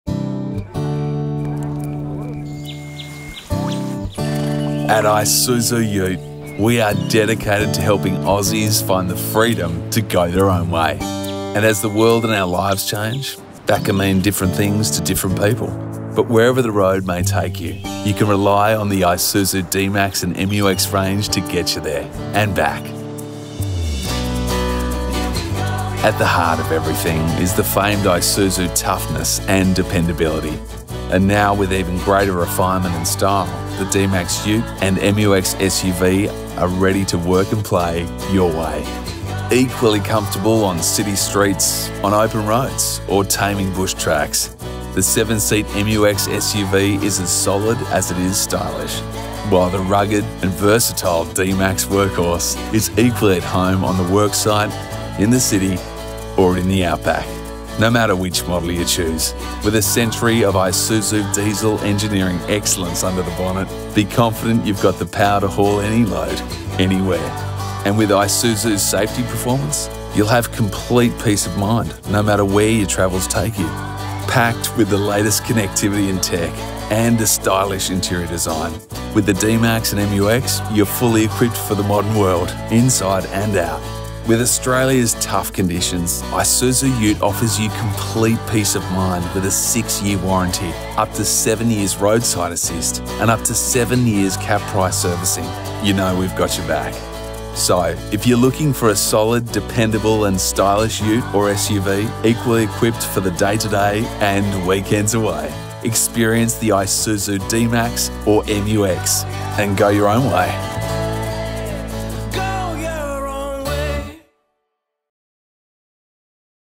The Isuzu Story – GO YOUR OWN WAY | Isuzu UTE Australia
At Isuzu UTE, we are dedicated to helping Aussies find the freedom to go their own way, and as the world and our lives change, that can mean different things to different people, but wherever the road may take you, you can rely on the Isuzu D-MAX and MU-X range to get you there and back.

At the heart of everything is the famed Isuzu toughness and dependability… and now, with even greater refinement and style, the D-MAX ute and MU-X SUV are ready to work and play your way. Equally comfortable on city streets, on open roads, or taming bush tracks, the seven-seat MU-X SUV is as solid as it is stylish. While the rugged and versatile D-MAX workhorse is equally at home on the worksite, in the city, or in the outback

Discover the Isuzu D-MAX and MU-X range today!